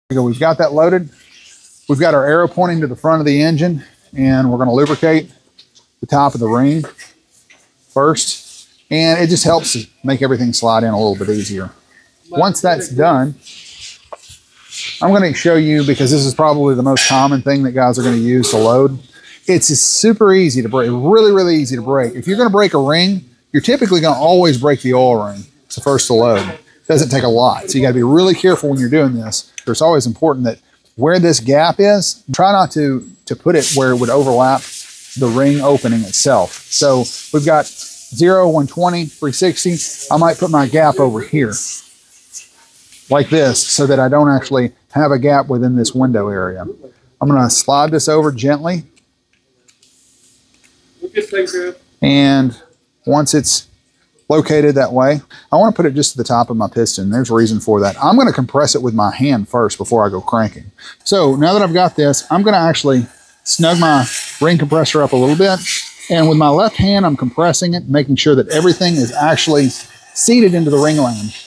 Installing Upgraded Pistons - Watch Those Gaps!
Cass gives us some tips and tricks on how to properly install upgraded pistons in a 6.7 Powerstroke engine. Looking for an upgraded set of pistons?